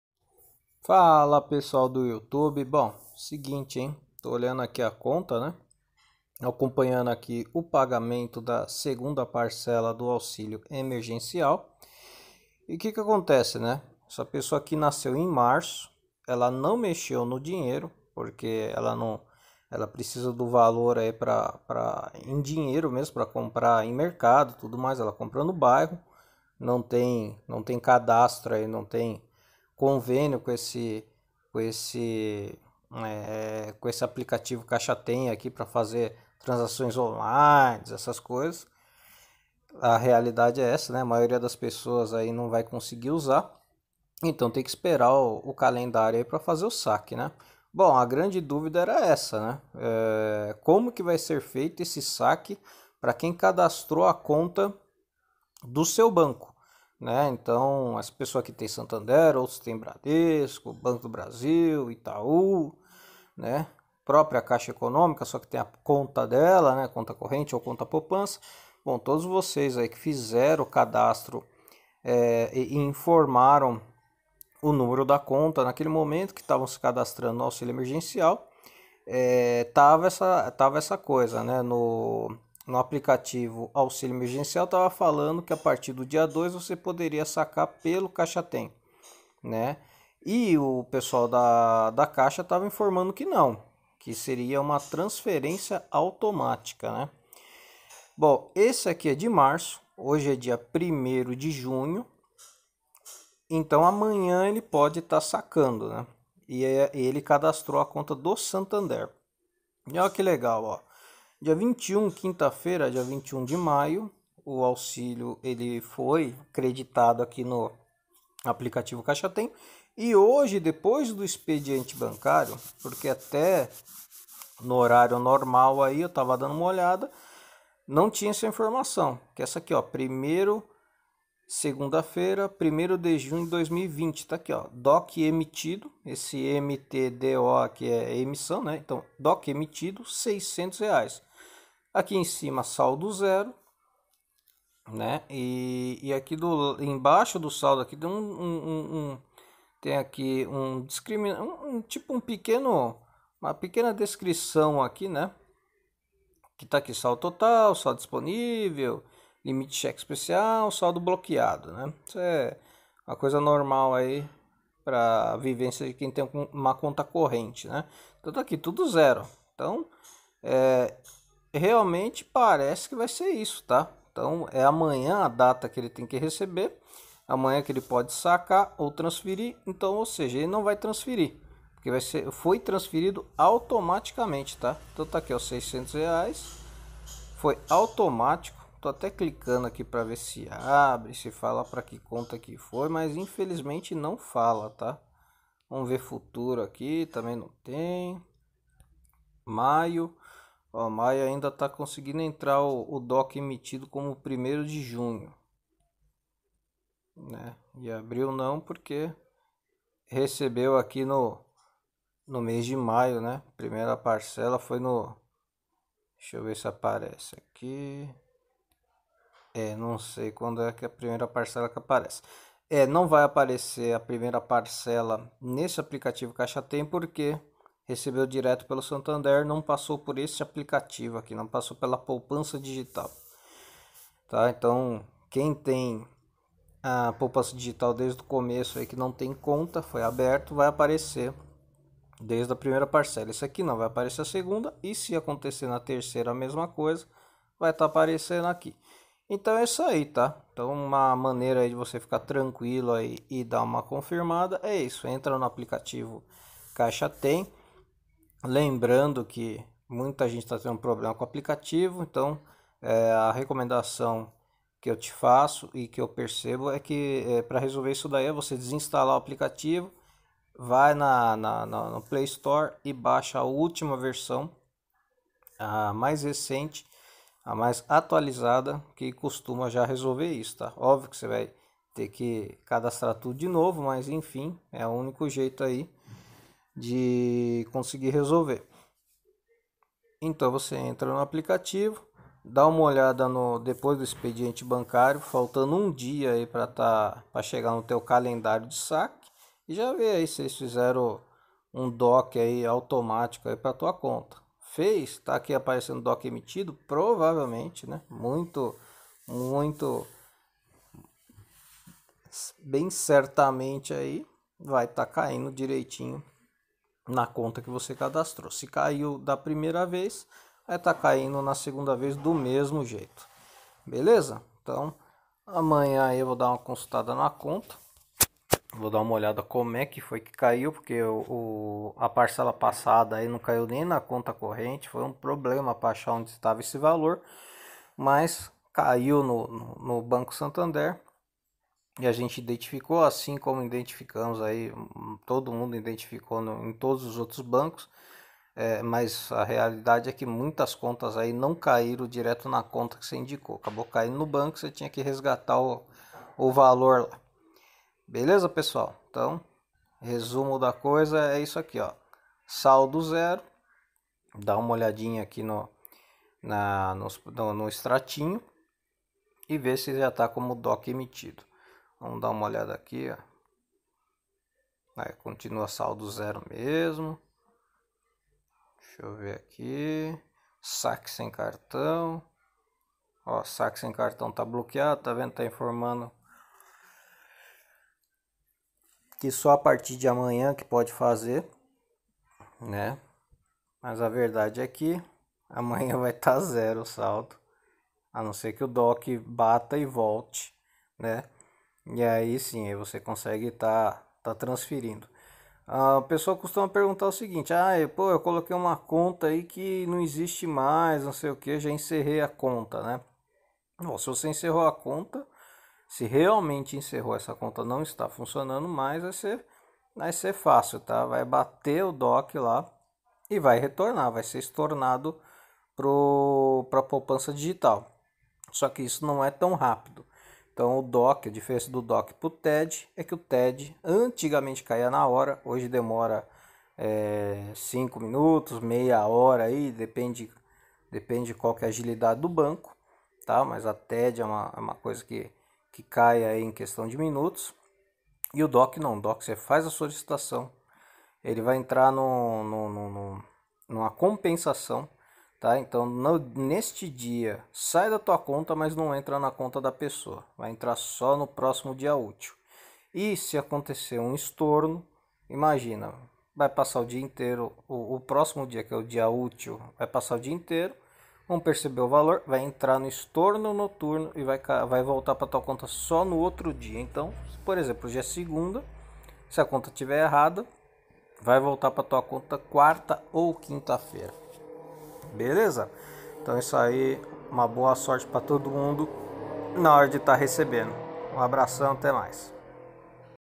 AUXÍLIO EMERGENCIAL NÃO CAIU NA MINHA CONTA: DESCOBRIMOS SE VAI NA CAIXA OU NO SEU BANCO 2ª PARCELA!
SEGUNDA PARCELA DO AUXÍLIO EMERGENCIAL R$600, R$1200:
CADASTROU SUA CONTA PARTICULAR: SAIBA SE A TRANSFERÊNCIA OU SAQUE, SERÁ PELA POUPANÇA DIGITAL CAIXA APLICATIVO CAIXA TEM OU PELO BANCO CADASTRADO NO APP AUXÍLIO EMERGENCIAL